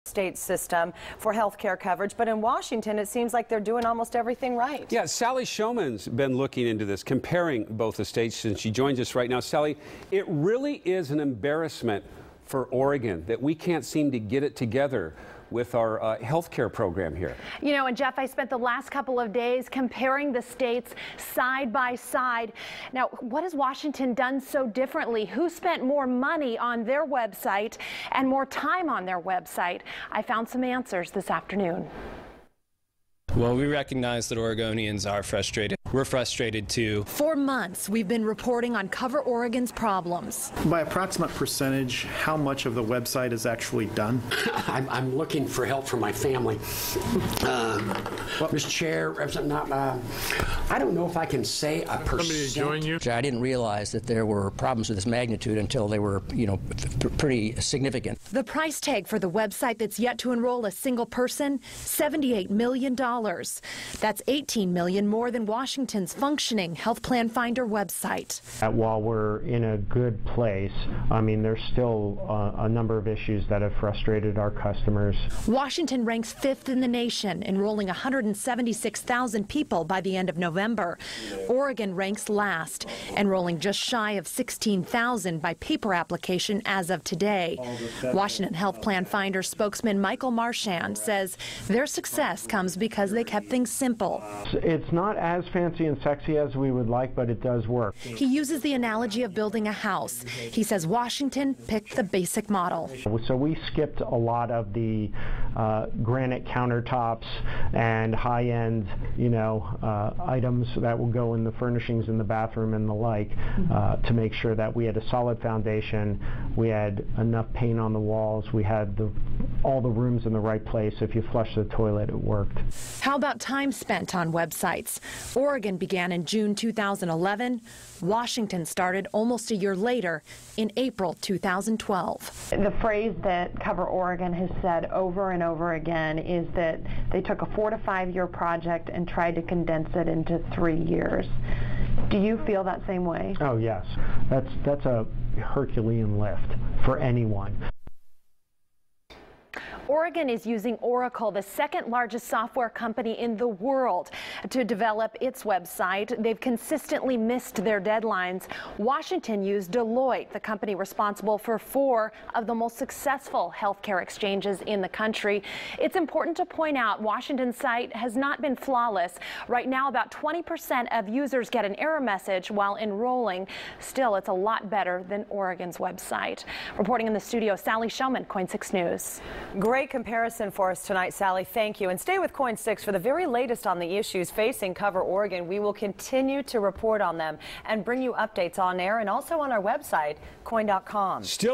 Health care numbers: WA trounces OR
Washington is 5th in the nation, Oregon is last.